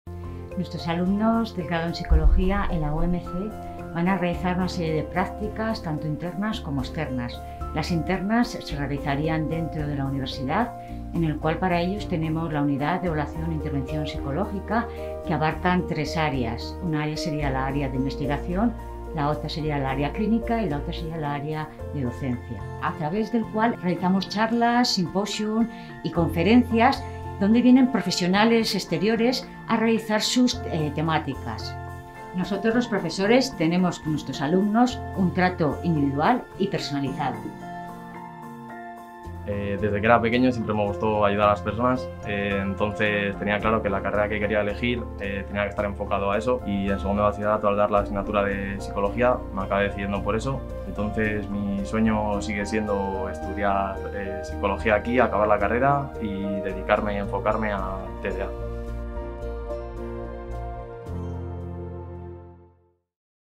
UEMC - Grado en Psicología
El grado en Psicología de la UEMC ofrece todos los conocimientos científicos necesarios para analizar y comprender el comportamiento humano. Todo ello de la mano de los mejores profesionales y con un acompañamiento personalizado de cada estudiante.

La UEMC dispone distintas instalaciones propias como una cámara Gesell y una docimoteca, equipadas con las últimas tecnologías, que facilitan el desarrollo de competencias profesionales de los alumnos.

La formación se complementa con prácticas internas y externas, lo que posibilita al estudiante a enfrentarse a casos reales.